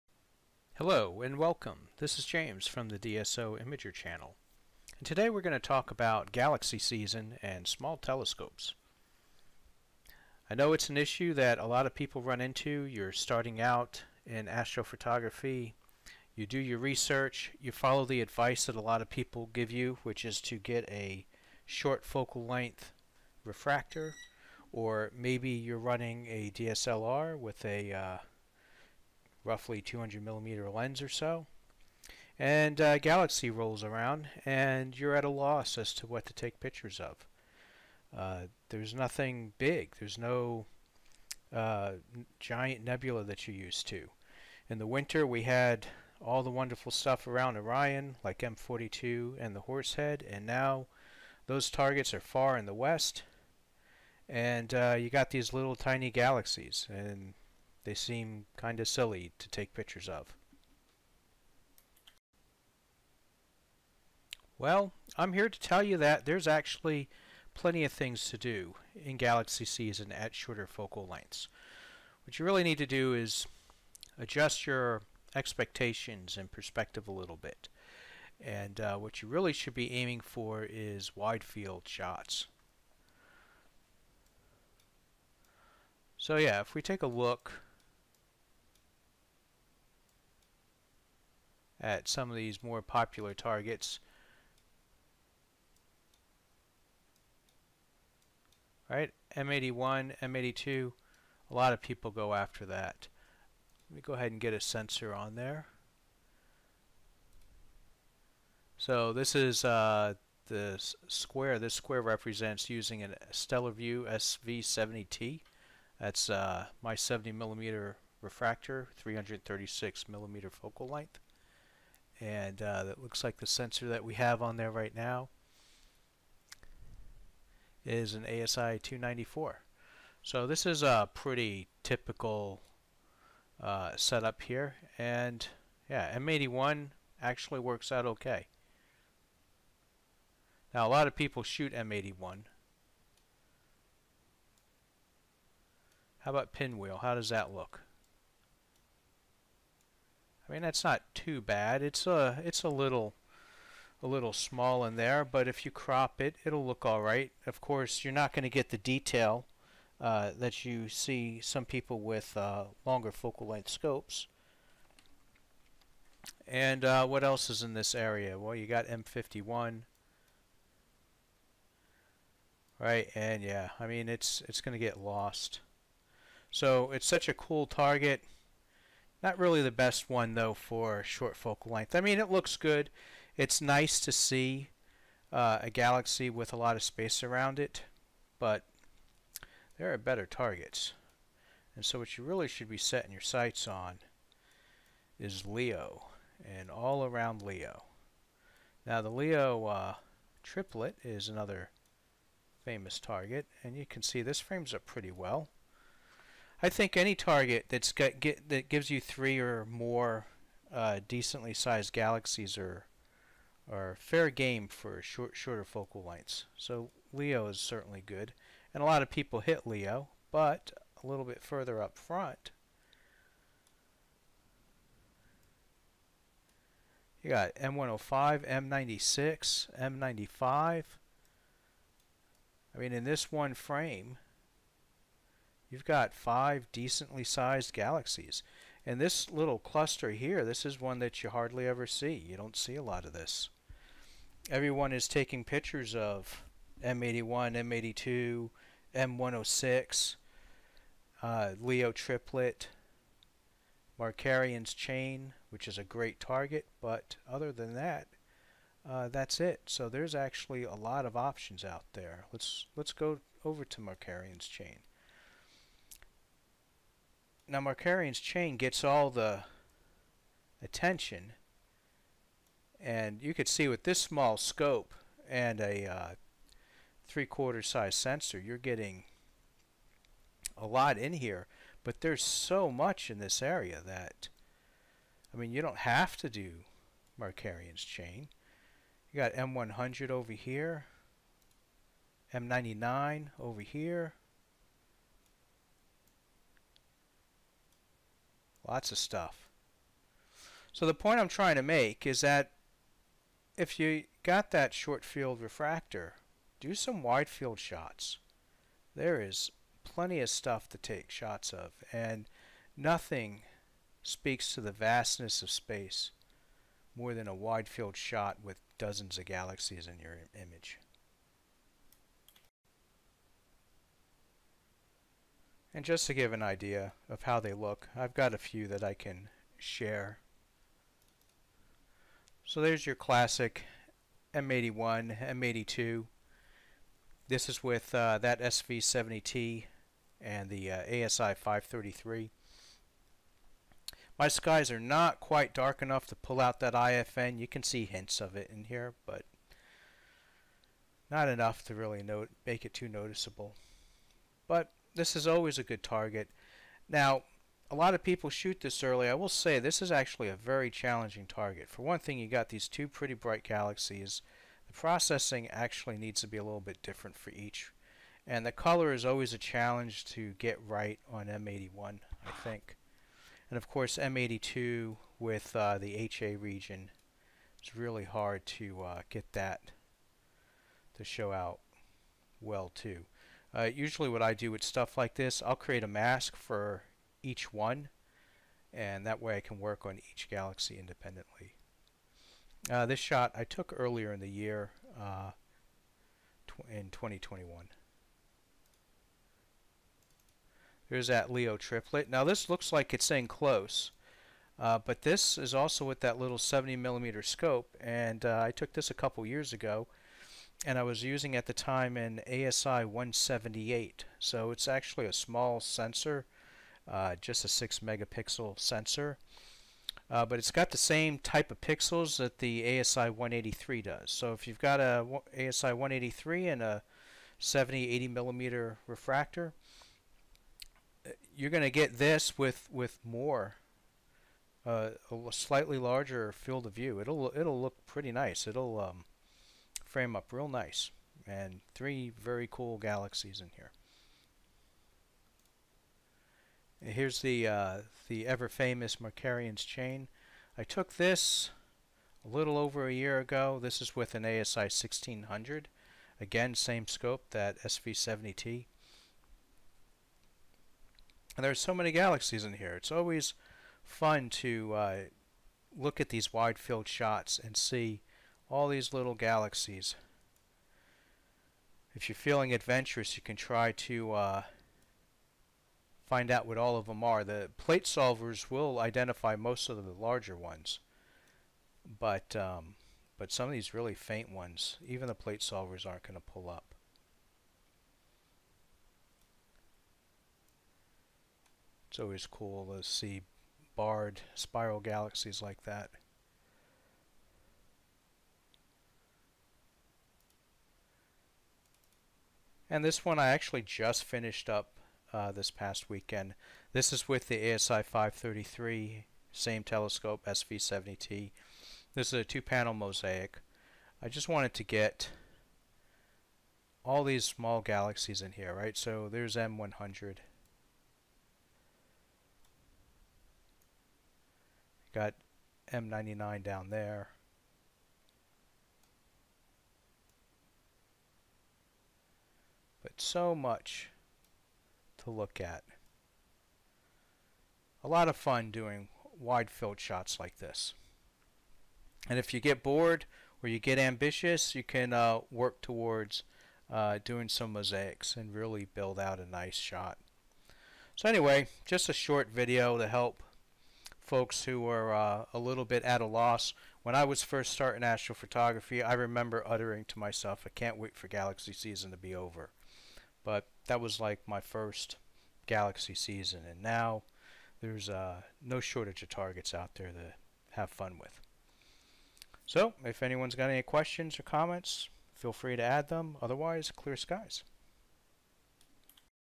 Galaxy Season with a small telescope.
Just because you are running a small telescope with a short focal length doesn't mean you can't have fun during galaxy season with backyard astrophotography!

Equipment used for the images taken in this video:

Mount:  Celestron AVX
Filters:  ZWO Luminance filter